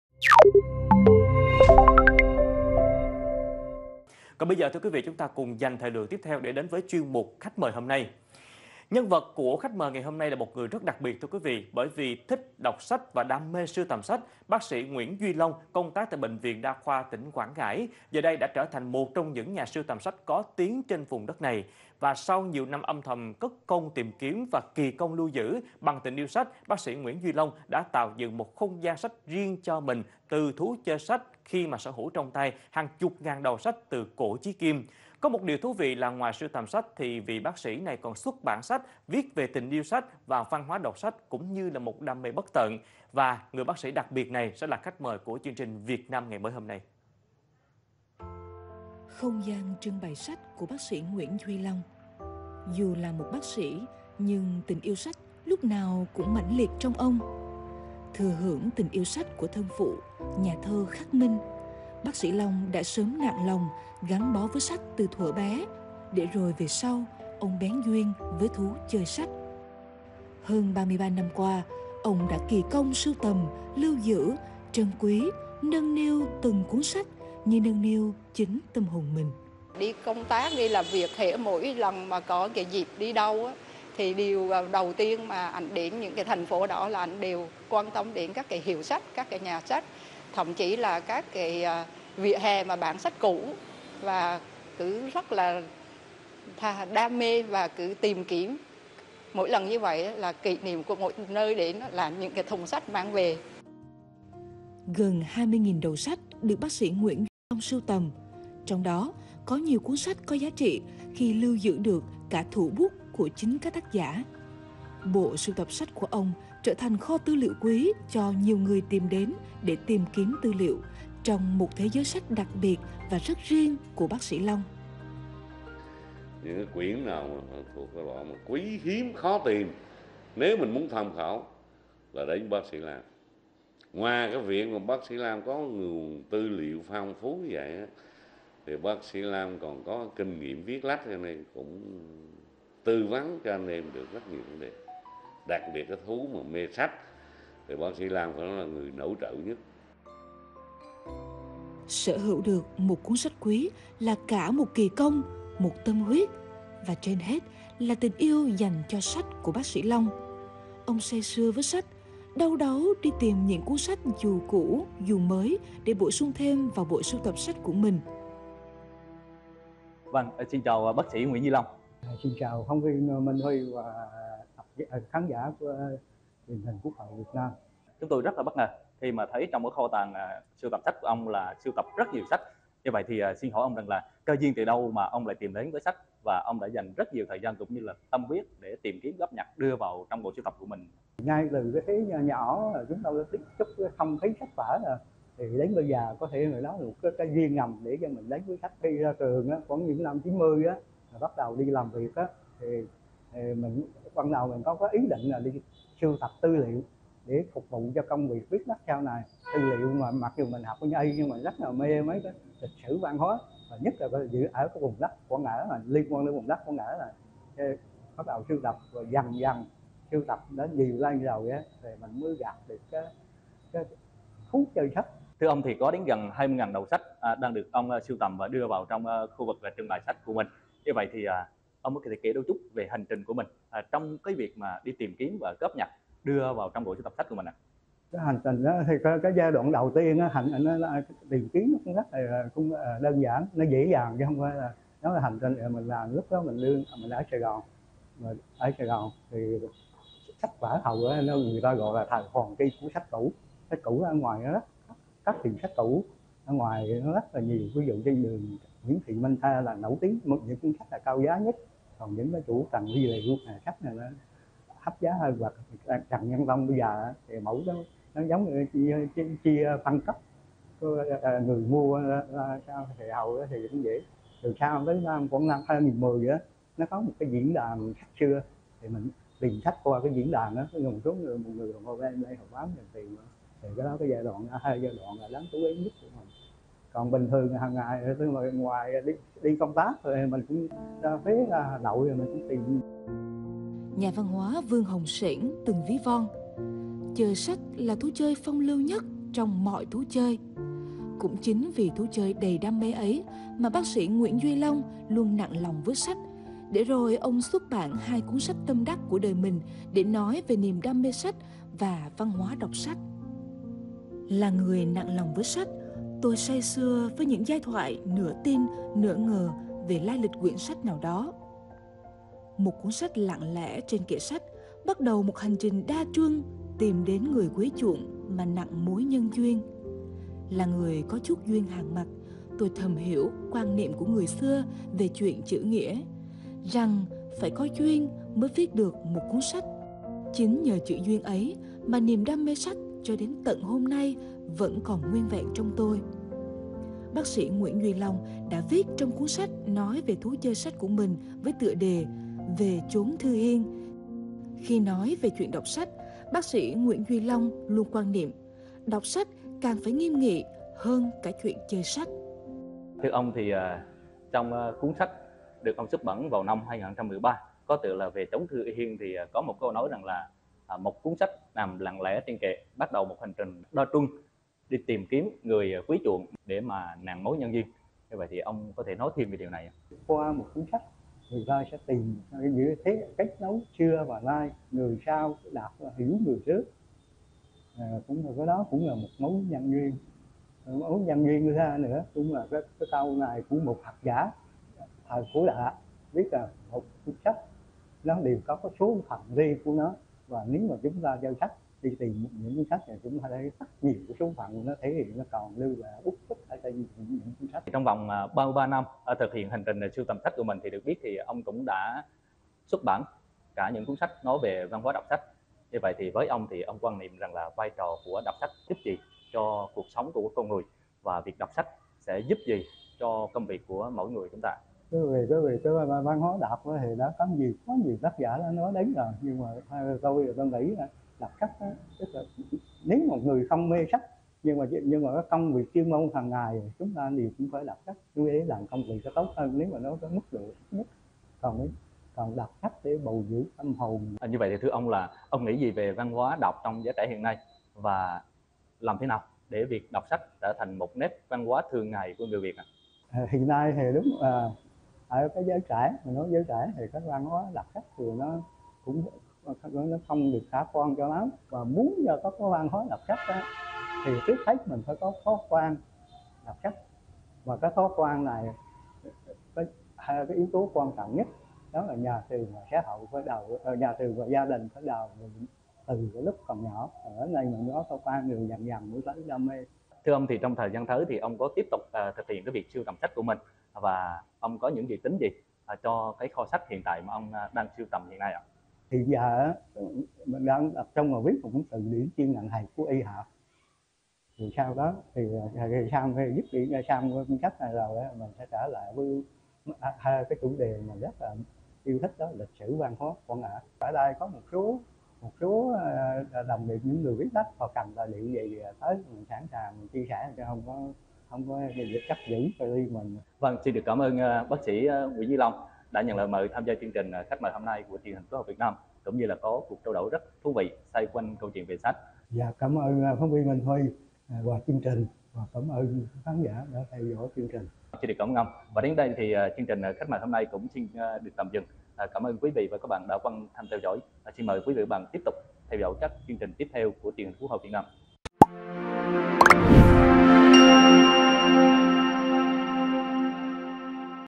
Vị bác sĩ đa khoa với 33 năm đam mê sưu tầm sách | Truyền hình Quốc hội Việt Nam
Truyền hình Quốc hội Việt Nam | Thích đọc sách và đam mê sưu tầm sách, bác sĩ Nguyễn Duy Long, công tác tại Bệnh viện đa khoa Quảng Ngãi, giờ đây đã trở thành một trong những nhà sưu tầm sách có tiếng tại tỉnh. Sau nhiều năm âm thầm cất công tìm kiếm và kỳ công lưu giữ, bác sĩ Nguyễn Duy Long đã tạo dựng một không gian sách riêng cho mình từ thú chơi sách khi sở hữu trong tay hàng chục ngàn đầu sách từ cổ chí kim.